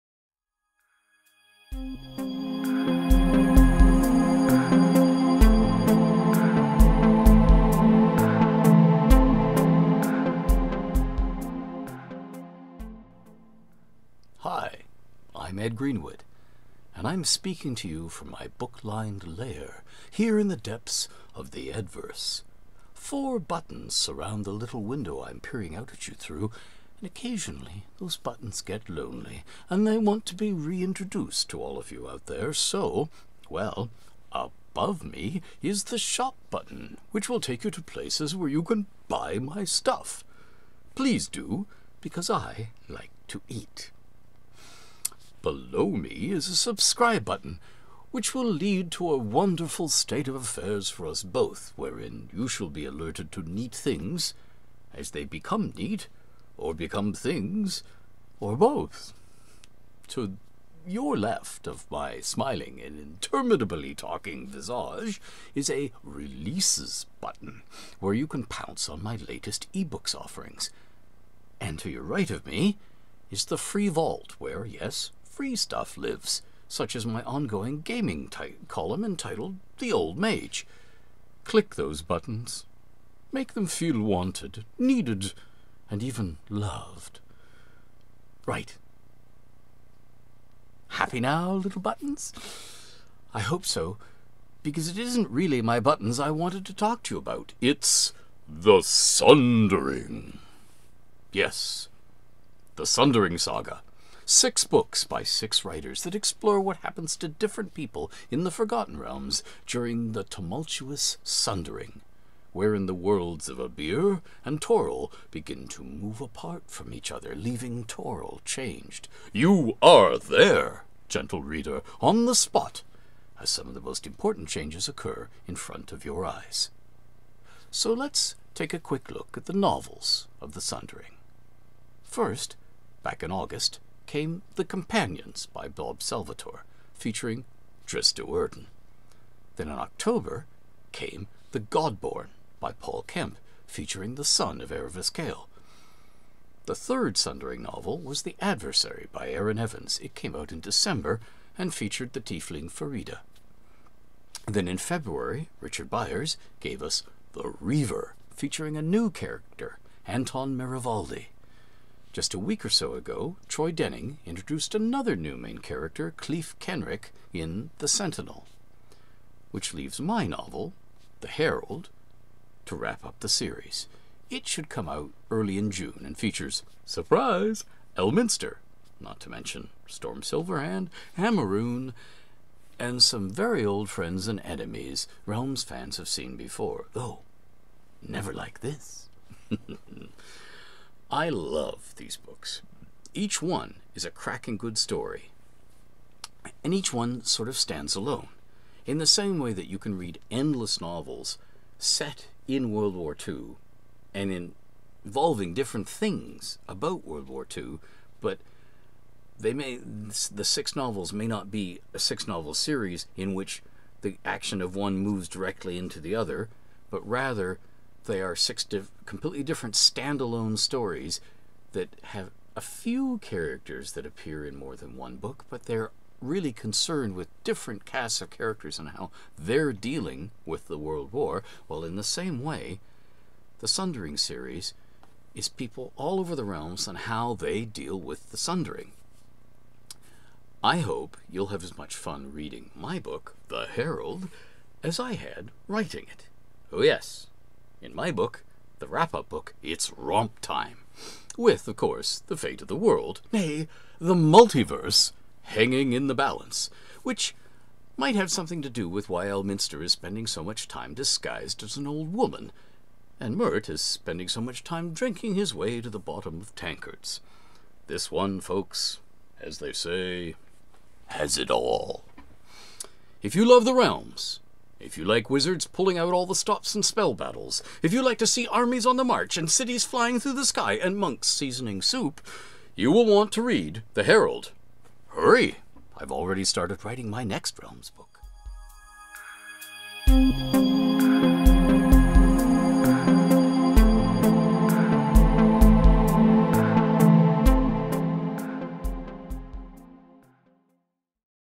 TheEdVerse | Blog | Home Update | The Sundering Saga | 17.04.2014
[Is Ed talking about his buttons again? That's because this video is meant to be viewed here: TheEdVerse.com  :)  ]

Ed Talks about the Sundering Saga.
The Sundering is the event that ends the Spellplague, that utter chaos of magic possibly precipitated by the murder of Mystra, Goddess of Magic and possibly by the world of Abeir crashing into and starting to pass through the world of Toril, home of the Forgotten Realms.®